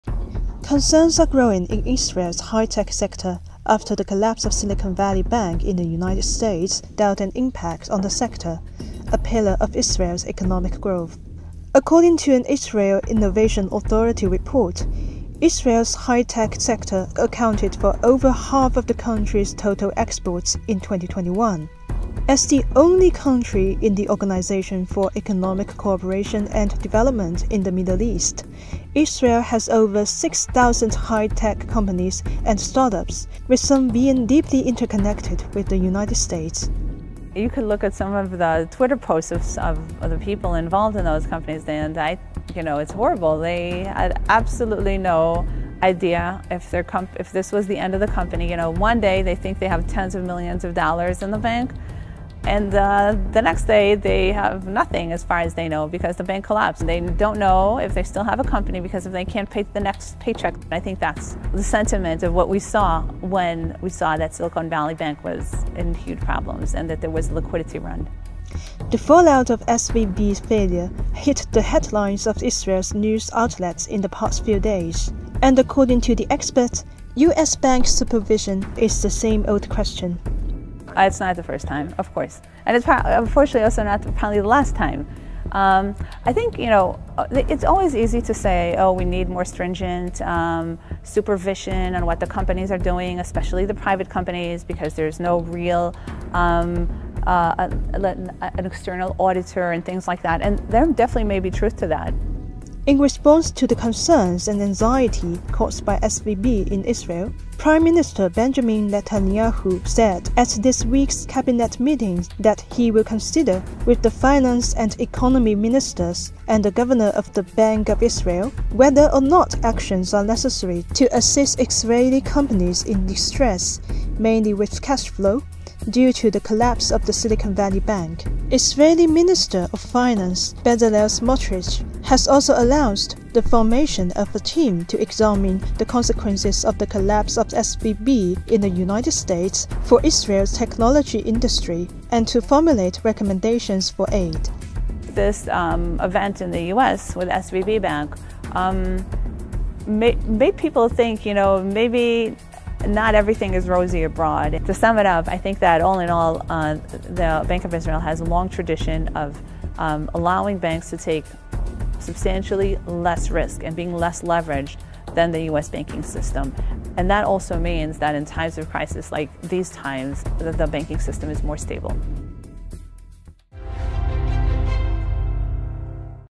GLOBALink | Concerns grow in Israel's high-tech sector amid SVB crisis
Concerns are growing in Israel's high-tech sector, after the collapse of Silicon Valley Bank (SVB) in the United States dealt an impact on the sector, a pillar of Israel's economic growth.